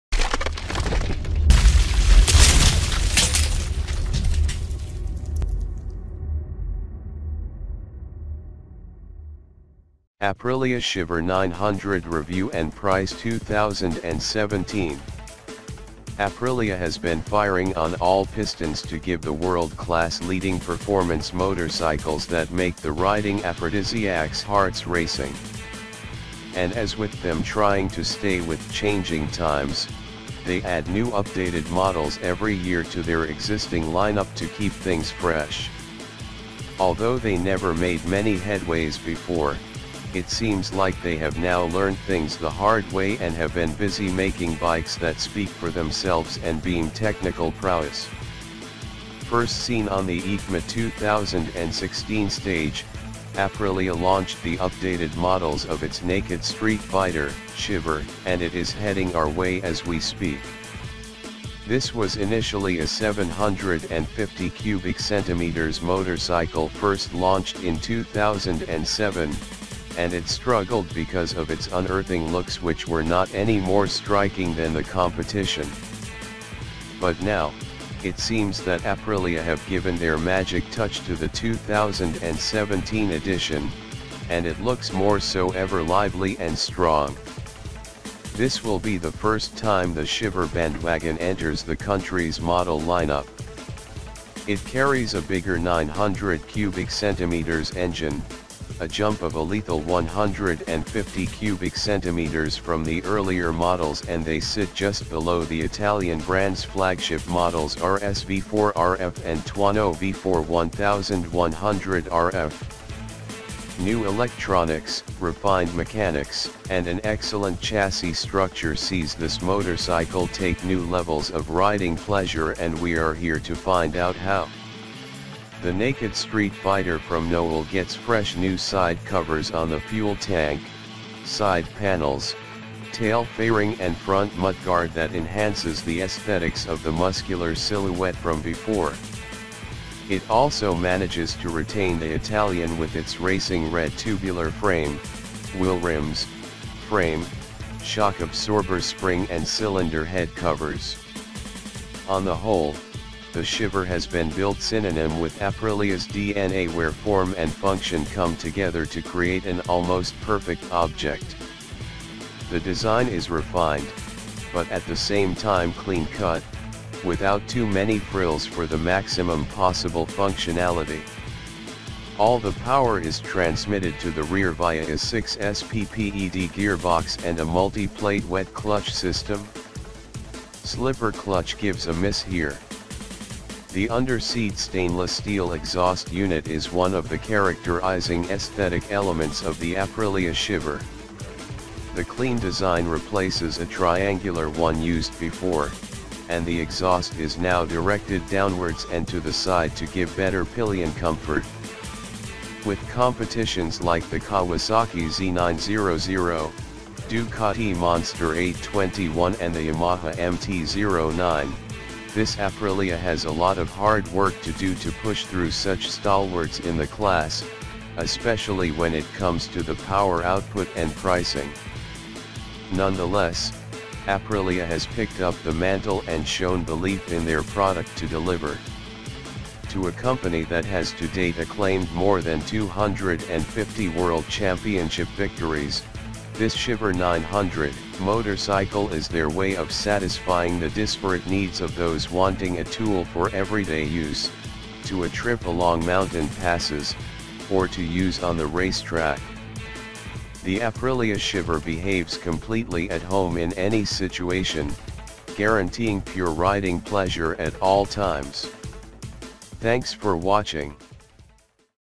Aprilia Shiver 900 Review and Price 2017
Nonetheless, Aprilia has picked up the mantle and shown belief in their product to deliver. To a company that has to date acclaimed more than 250 world championship victories, this Shiver 900 motorcycle is their way of satisfying the disparate needs of those wanting a tool for everyday use, to a trip along mountain passes, or to use on the racetrack. The Aprilia Shiver behaves completely at home in any situation, guaranteeing pure riding pleasure at all times.

2017 Aprilia Shiver 900 Review, 2017 Aprilia Shiver 900 Price,